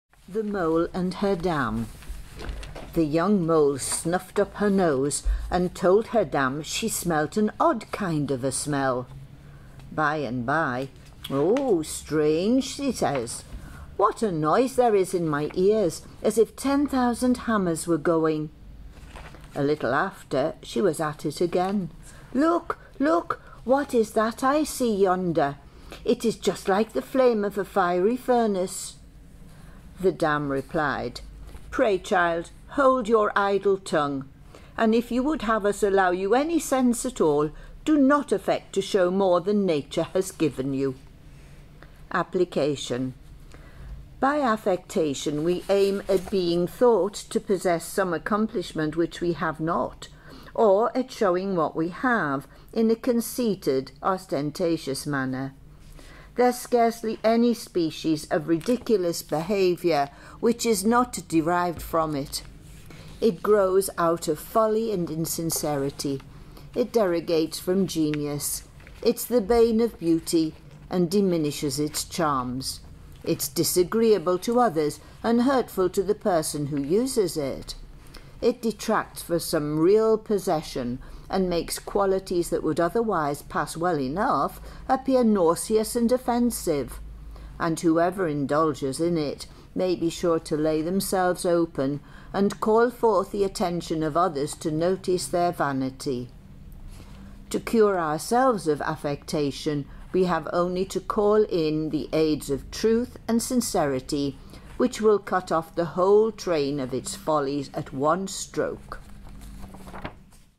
The Mole and Her Dam
From the Fables of Aesop and Others with Designs on Wood by Thomas Bewick, 1818. Read in Newcastle upon Tyne by members of Explore For more information on Thomas Bewick (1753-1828) and his appprentices